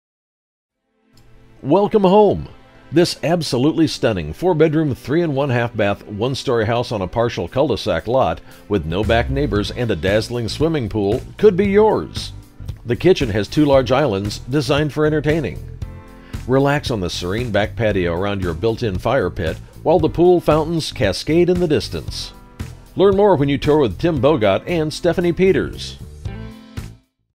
2603 Park Hills Drive Katy, TX | ColdwellBankerHomes.com
2603 Park Hills Drive, Katy, TX

Welcome home! This absolutely STUNNING four bedroom, three and one-half bath, ONE STORY on a partial cul-de-sac lot with NO BACK NEIGHBORS and a DAZZLING SWIMMING POOL could be yours! Wood floors, white cabinets, stainless steel appliances, granite countertops everywhere and high ceilings are only a